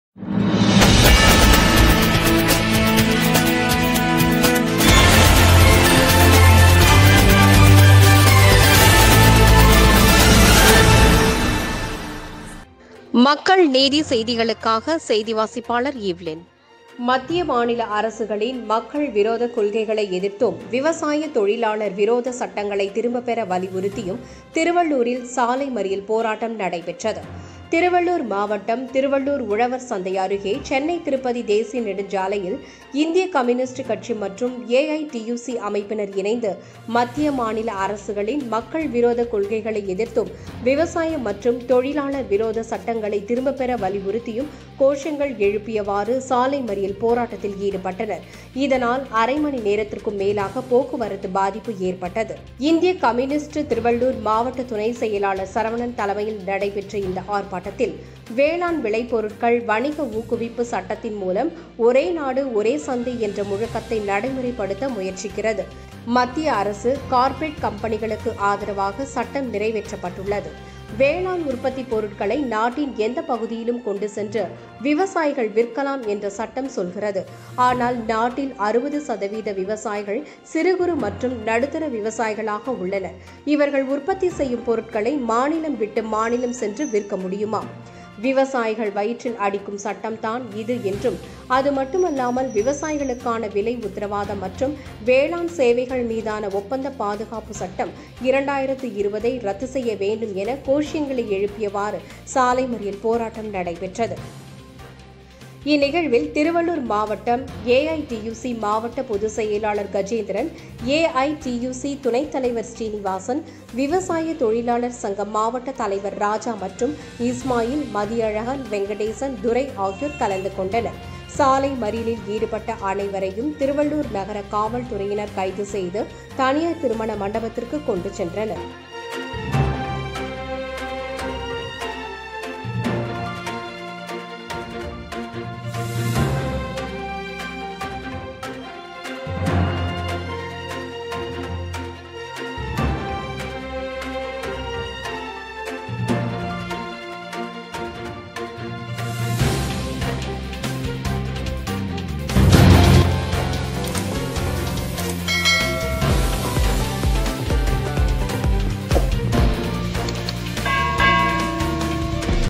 AITUC I CPI மத்திய மாநில அரசை கண்டித்து திருவள்ளூரில் ஆர்ப்பாட்டம்
AITUC I IC மத்திய மாநில அரசை கண்டித்து இந்திய கம்யூனிஸ்ட் மற்றும் ஏஐடியூசி திருவள்ளூரில் ஆர்ப்பாட்டம் @makkalneethitv